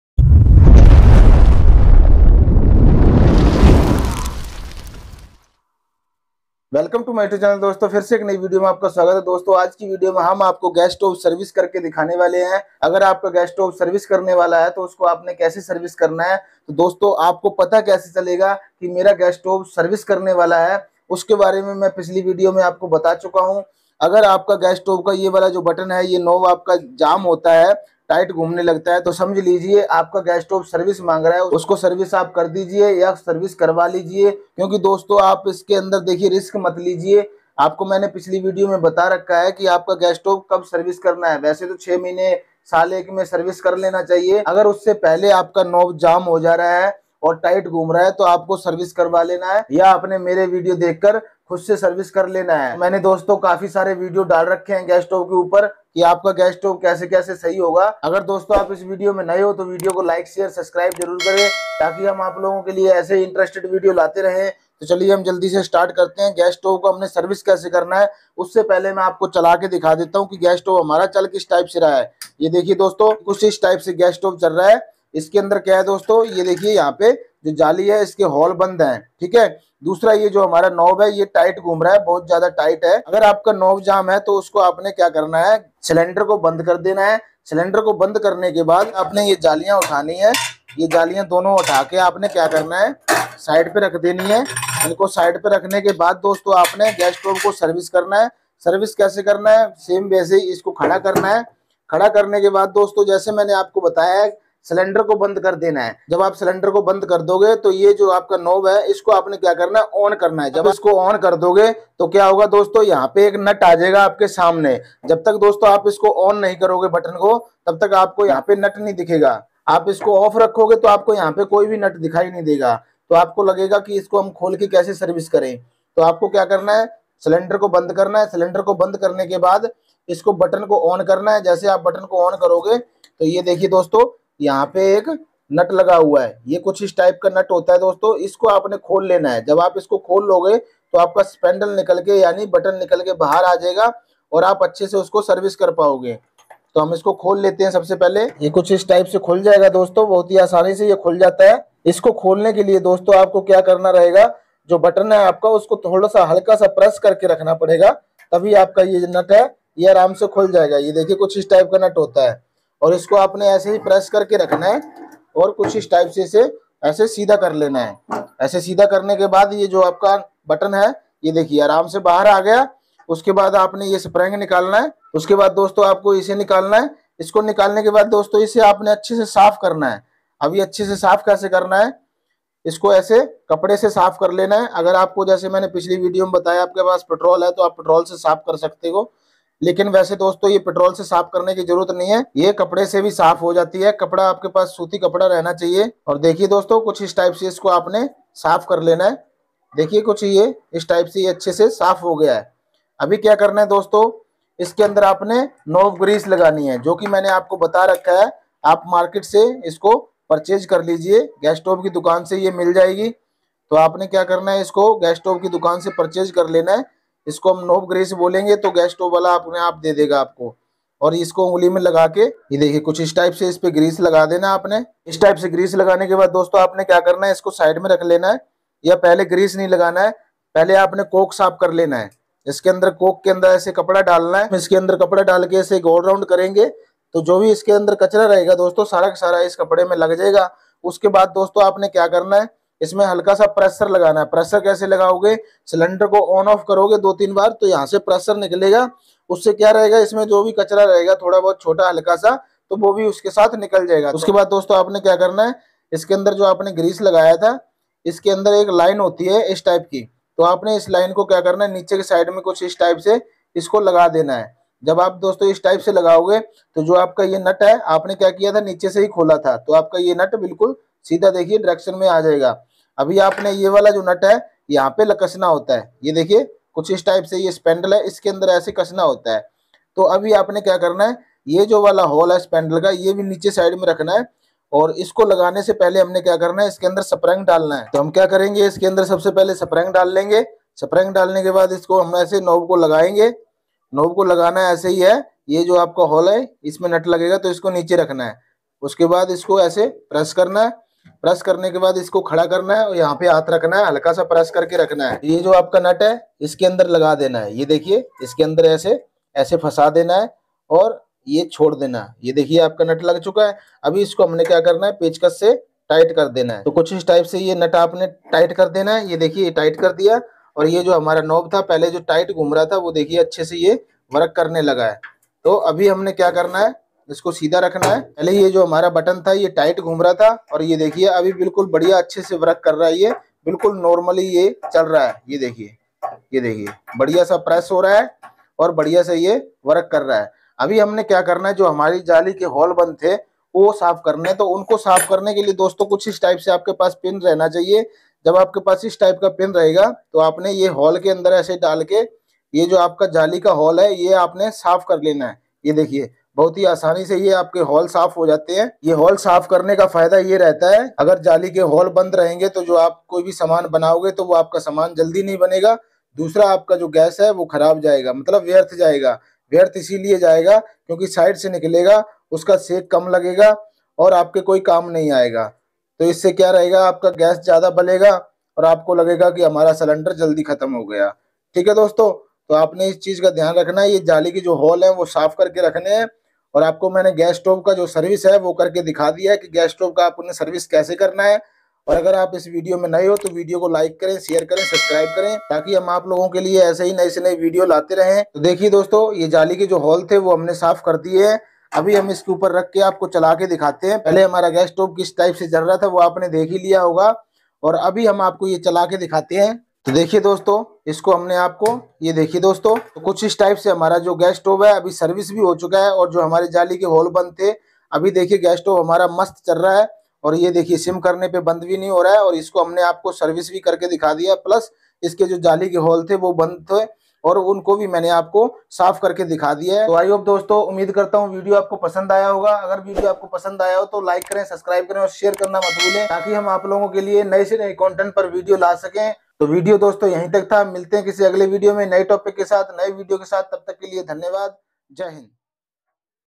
Gas stove service at home | gas chulha service kaise karen | gas stove repair
Dosto aaj ki video me humne aapko gas stove kaise service karna hai or kab service karna hai sabhi jankari and service karna sikhaya hai

Dosto Ye Sabhi Product Me Use Karta Hu Acchi Quality Ke Hai Aap Bhi Use Kar Sakte Ho Dhanyvad:-

Refer Code= YYEMO3759190